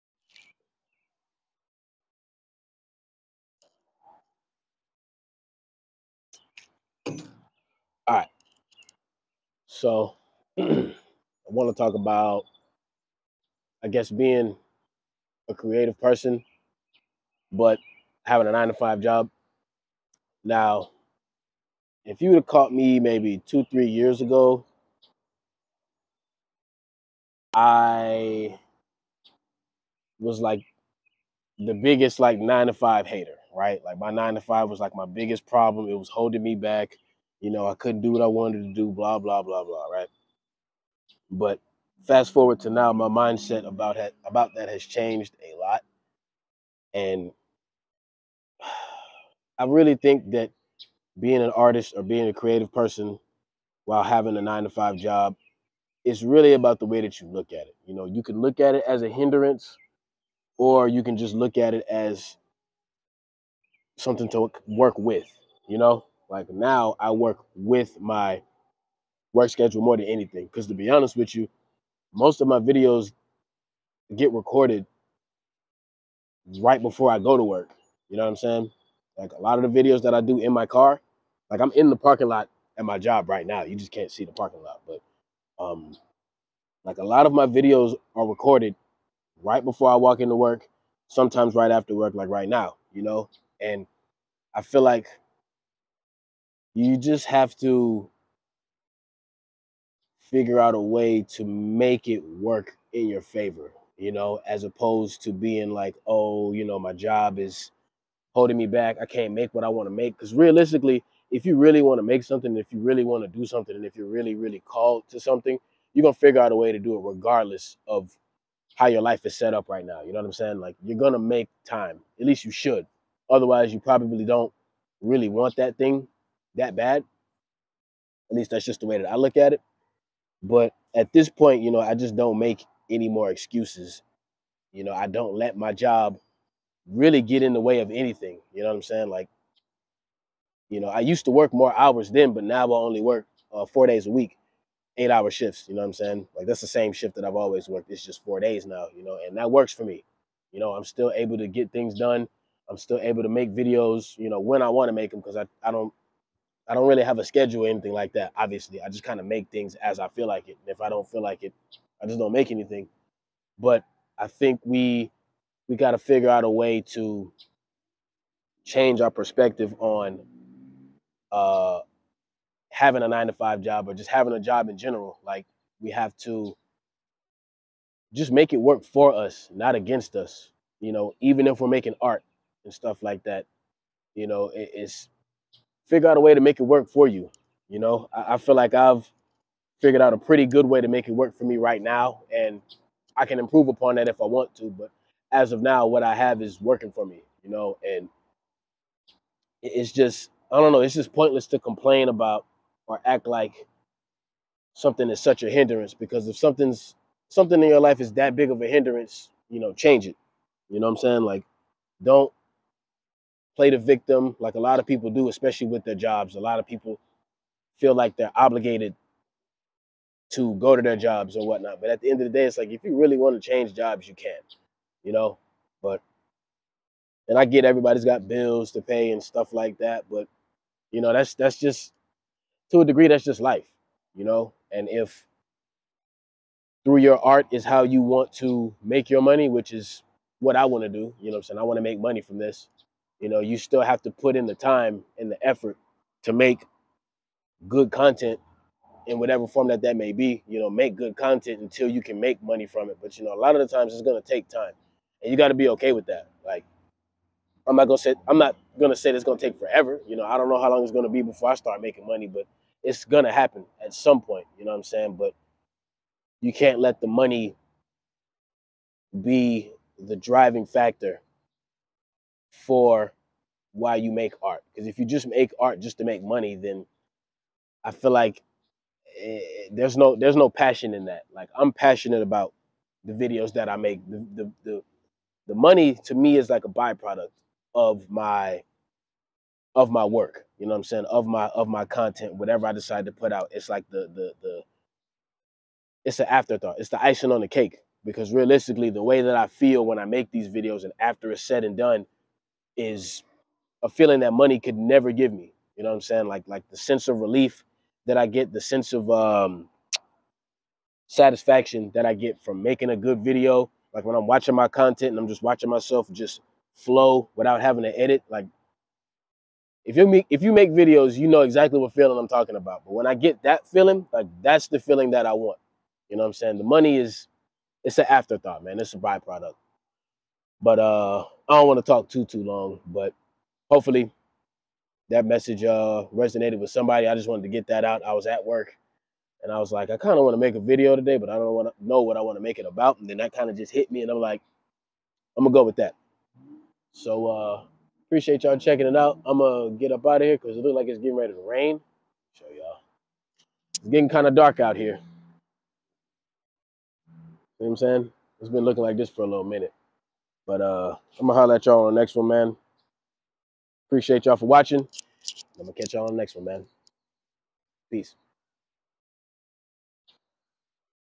Having a 9 to 5 job and being a creative/ artist
From a former 9 to 5 hater, it's not so  bad. Perspective matters